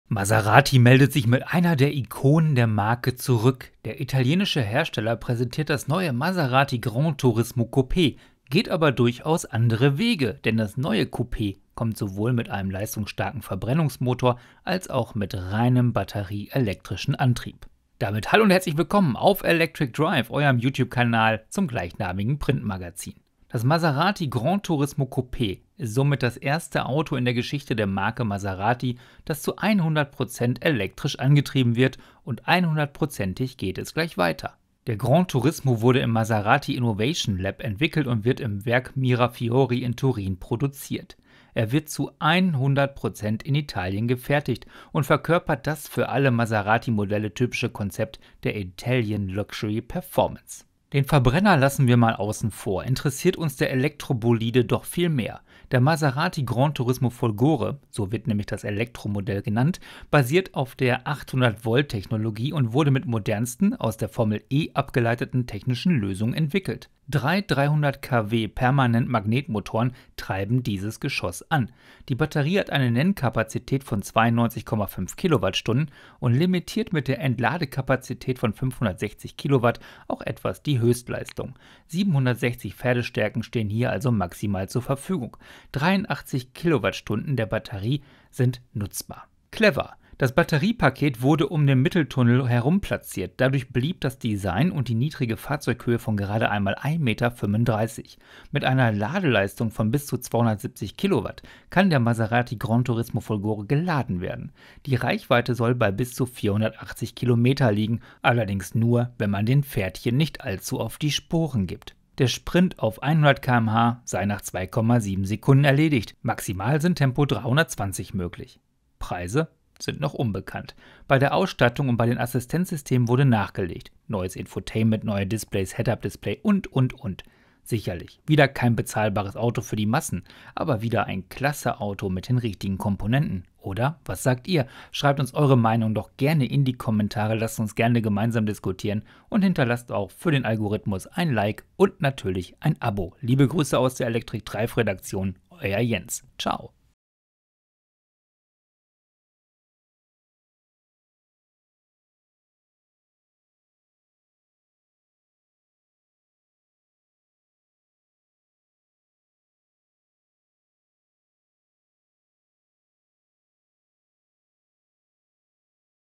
2023 Maserati GranTurismo Folgore | technische Daten Reichweite Leistung | Electric Drive News
Maserati meldet sich mit einer der Ikonen der Marke zurück, der italienische Hersteller präsentiert das neue Maserati GranTurismo Coupé, geht aber durchaus andere Wege, denn das neue Coupé kommt sowohl mit einem leistungsstarken Verbrennungsmotor als auch mit reinem batterieelektrischen Antrieb. Das Maserati GranTurismo Coupé ist somit das erste Auto in der Geschichte der Marke Maserati, das zu 100 Prozent elektrisch angetrieben wird und 100 prozentig geht es gleich weiter.

Der GranTurismo wurde im Maserati Innovation Lab entwickelt und wird im Werk Mirafiori in Turin produziert. Er wird zu 100 Prozent in Italien gefertigt und verkörpert das für alle Maserati Modelle typische Konzept der "Italian luxury performance".

Den Verbrenner lassen wir mal aussen vor, interessiert uns der Elektro-Bolide doch viel mehr. Der Maserati GranTurismo Folgore basiert auf der 800-Volt-Technologie und wurde mit modernsten, aus der Formel E abgeleiteten technischen Lösungen entwickelt. Drei 300-kW-Permanentmagnetmotoren treiben dieses Geschoss an, die Batterie hat eine Nennkapazität von 92,5 kWh und limitiert mit der Entladekapazität von 560 kW auch etwas die Höchstleistung. 760 Pferdestärken stehen hier also maximal zur Verfügung, 83 kWh der Batterie sind nutzbar.

Das Batteriepaket wurde um den Mitteltunneln herum platziert, dadurch blieb das Design und die niedrige Fahrzeughöhe von gerade einmal 1,35 Meter. Mit einer Ladeleistung von bis zu 270 kW kann der Maserati GranTurismo Folgore geladen werden, die Reichweite soll bei bis zu 480 Kilometer liegen, allerdings nur wenn man den Pferdchen nicht allzuoft die Sporen gibt. Der Sprint auf 100 km/h sei nach 2,7 Sekunden erledigt, maximal sind Tempo 320 km/h möglich.

Preise? Sind noch unbekannt, bei der Ausstattung und bei den Assistenzsystemen wurde nachgelegt, neues Infotainment, neue Displays, Head-up-Display und und und... sicherlich wieder kein bezahlbares Auto für die Massen, aber wieder ein klasse Auto mit den richtigen Komponenten, oder?